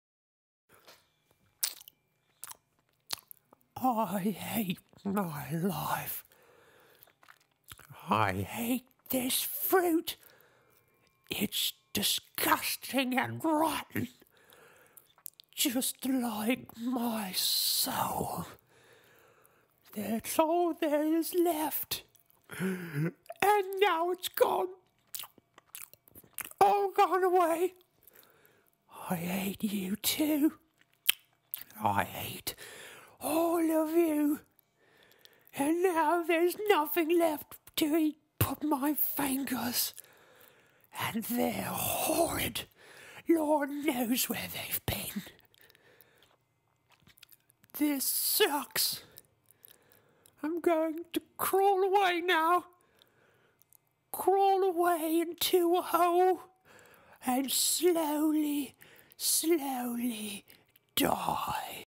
Lori the Loris
A very very depressed Loris, who thinks his life is much worse than it is. What's a Loris you ask? A really cute/sad looking animal. Don't be like this guy, be happy.